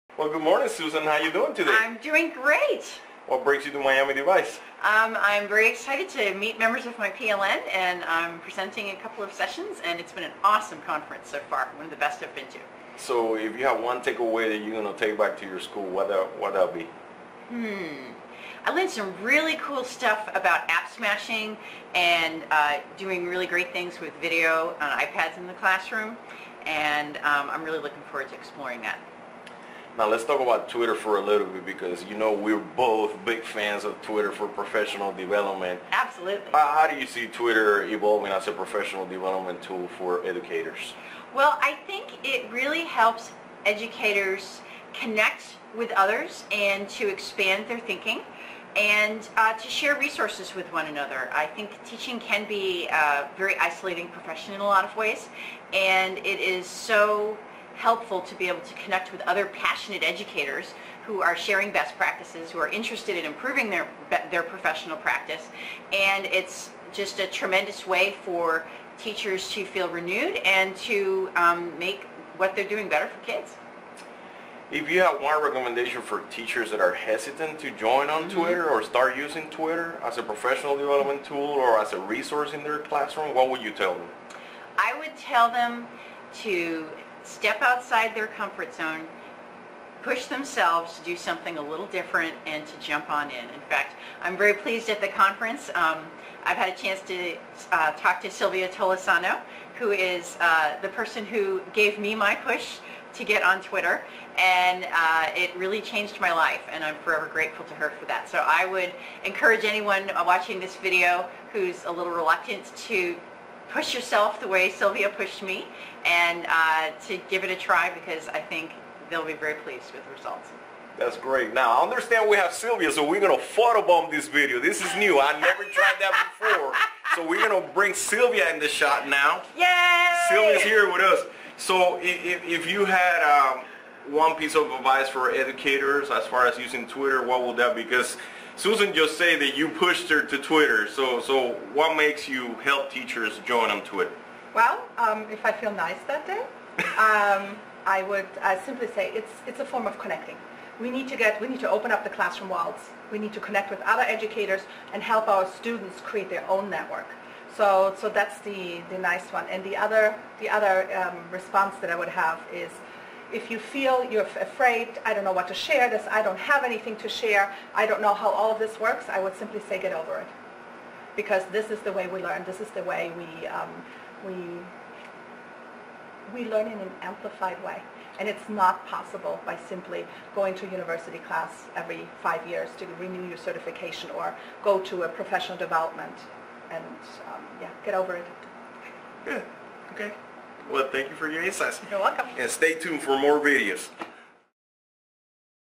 Twitter for Educators at MiamiDevice
Twitter for Educators at MiamiDevice with @s_bearden and @langwitches filmed on location at St. Stephen's Episcopal Day School.